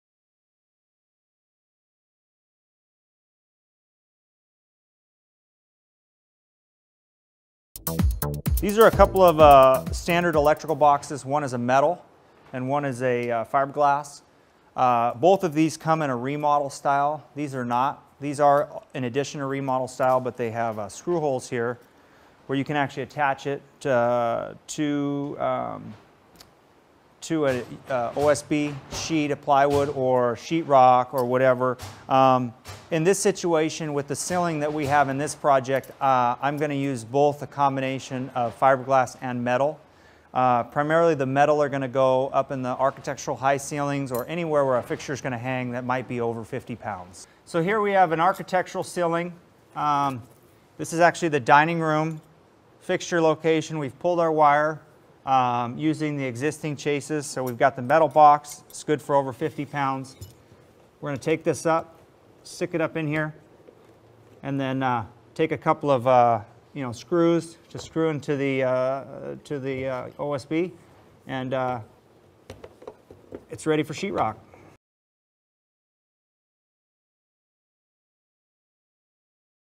Installing a ceiling box in a SIP roof panel
Enercept was the first SIP manufacturer to patent factory-installed electrical chases.  Our roof panels can be pre-wired in the factory with a limited number of electrical chases (when permitted by local building codes). Simply drill access holes through the sill and top plates for access to the electrical chases. A comprehensive Enercept Electrical Guide is available